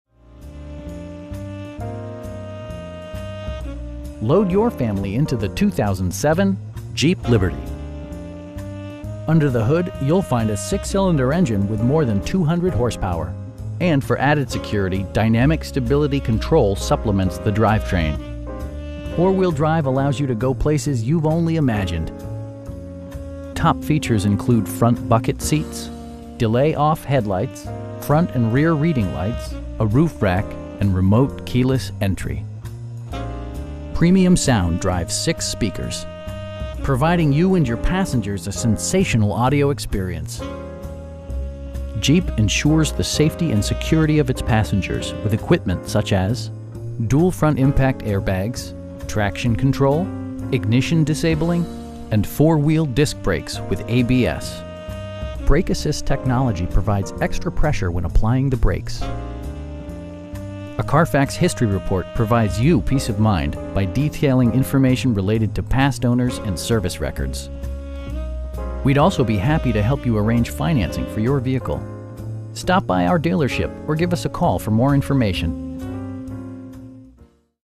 2007 Jeep Liberty SPORT in Fort Collins, CO 80525
Dellenbach Subaru
3103 S College Ave

Load your family into the 2007 Jeep Liberty. Under the hood you'll find a 6 cylinder engine with more than 200 horsepower, and for added security, dynamic Stability Control supplements the drivetrain. Four wheel drive allows you to go places you've only imagined. Top features include front bucket seats, delay-off headlights, front and rear reading lights, air conditioning, power door mirrors, power windows, a roof rack, and remote keyless entry. Premium sound drives 6 speakers, providing you and your passengers a sensational audio experience. Jeep ensures the safety and security of its passengers with equipment such as: dual front impact airbags, traction control, ignition disabling, and 4 wheel disc brakes with ABS. Brake assist technology provides extra pressure when applying the brakes. A Carfax history report provides you peace of mind by detailing information related to past owners and service records. We'd also be happy to help you arrange financing for your vehicle. Stop by our dealership or give us a call for more information.